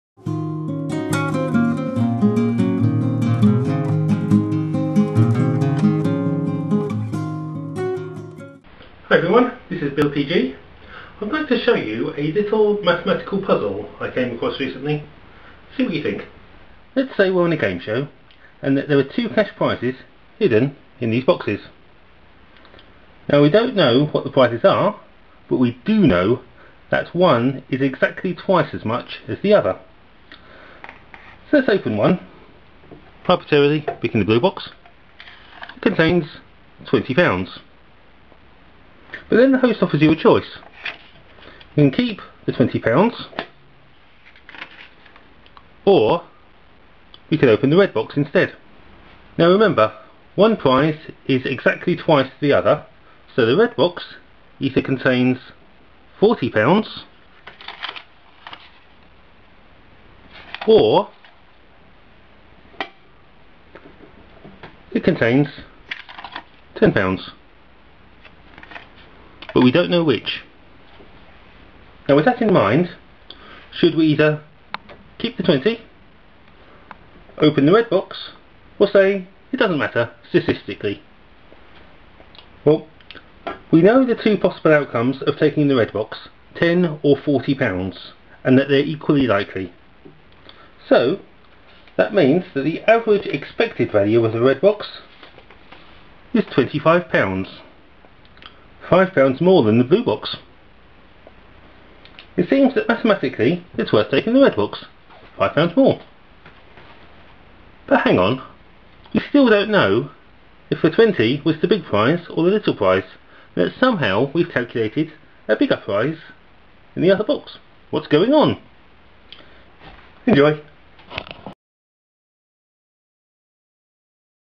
A mathematical puzzle...
Here's a game theory puzzle. Should you open the box?

I'm using the YT subtitles facility. You can switch them off using the button on the bottom right of the player.